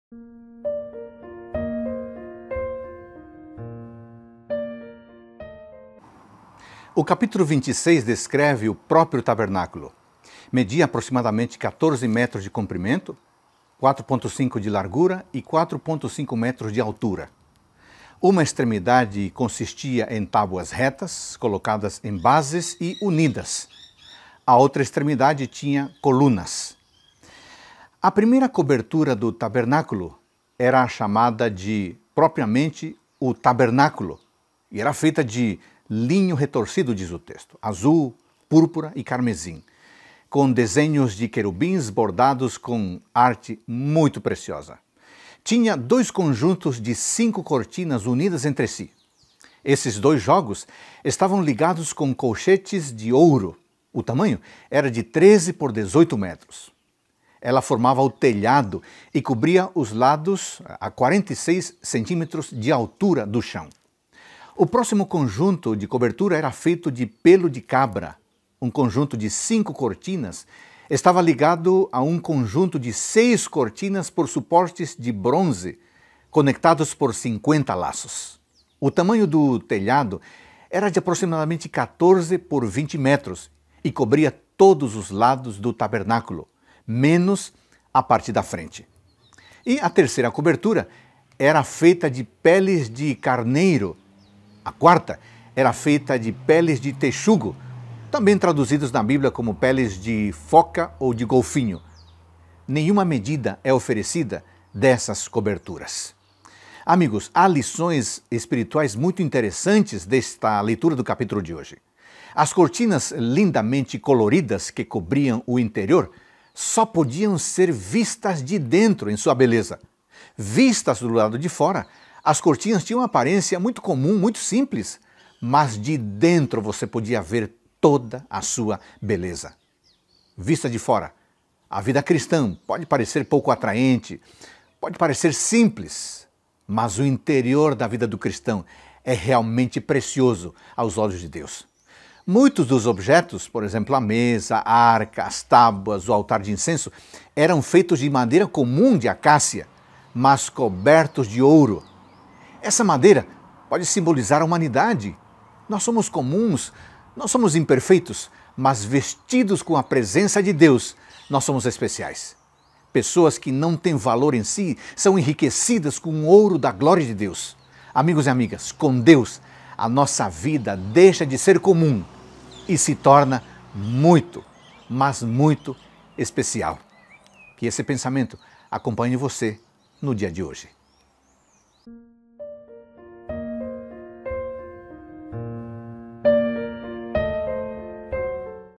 Êxodo 26 - Reavivados por Sua Palavra
Pessoas que não têm valor em si, são enriquecidas com o ouro da glória de Deus. Que maravilha: Com Deus, nossa vida deixa de ser comum e se torna especial!